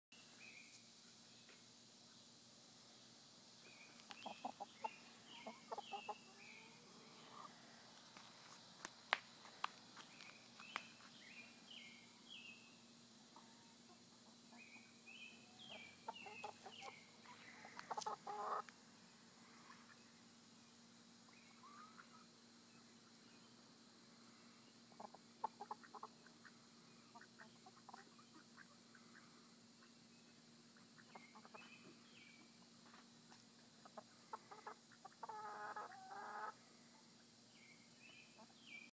Mass of caterpillars
A baseball sized mass of caterpillars outside my house.  Chickens won't touch them.